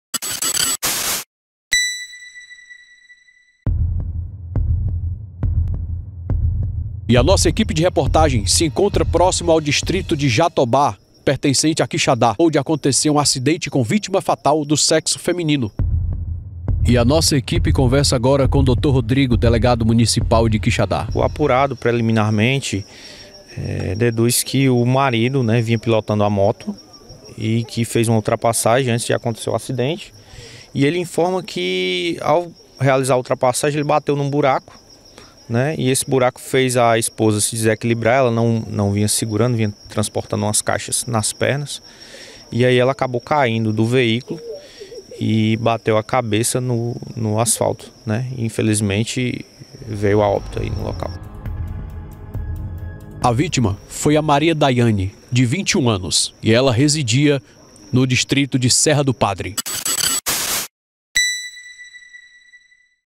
Quixadá Ce
Quixadá: mulher morre em acidente de trânsito, na tarde desta quinta-feira (14), na estrada que dá acesso ao distrito de Custódio, zona rural de Quixadá.